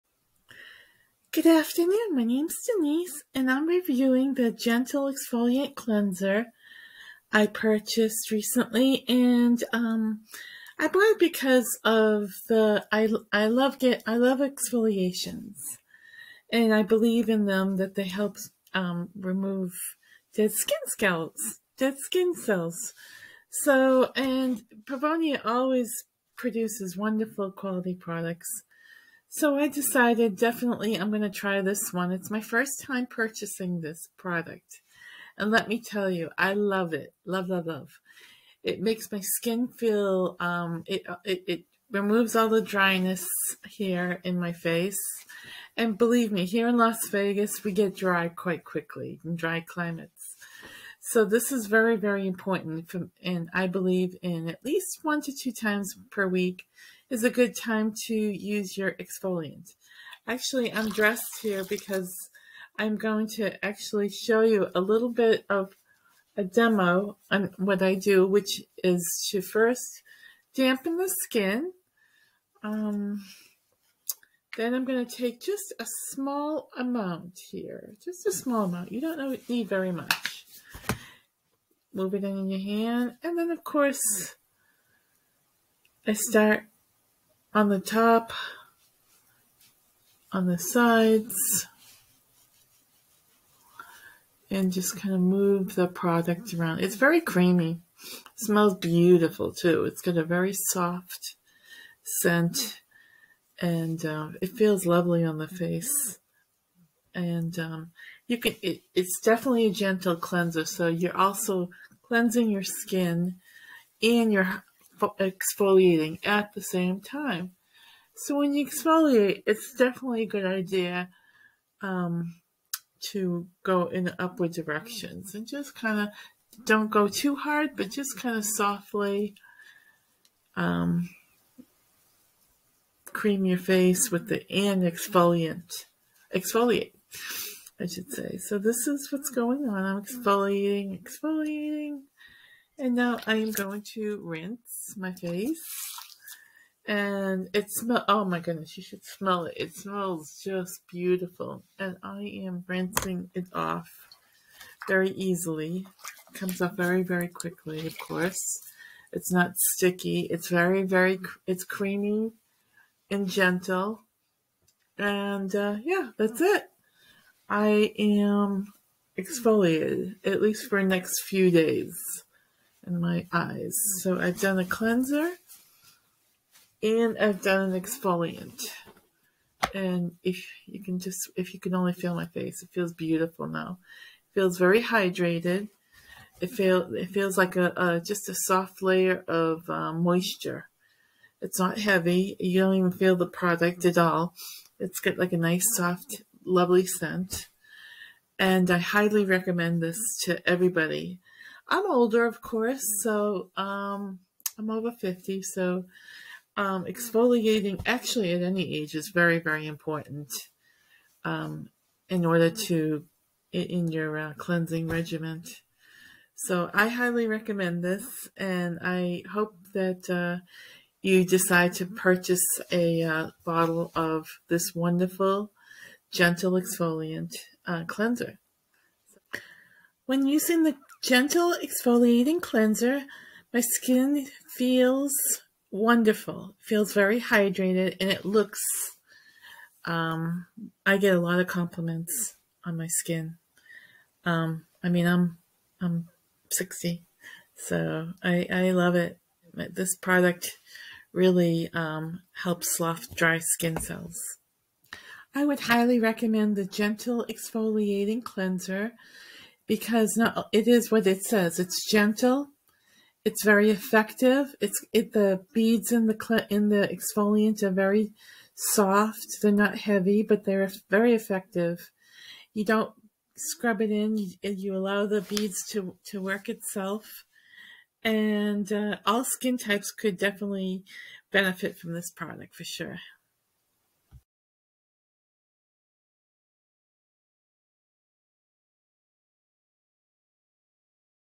Pevonia's Gentle Exfoliating Cleanser Review - Denise
Check out Denise's review of our Gentle Exfoliating Cleanser!
Shop for this product and the entire Special Care Line right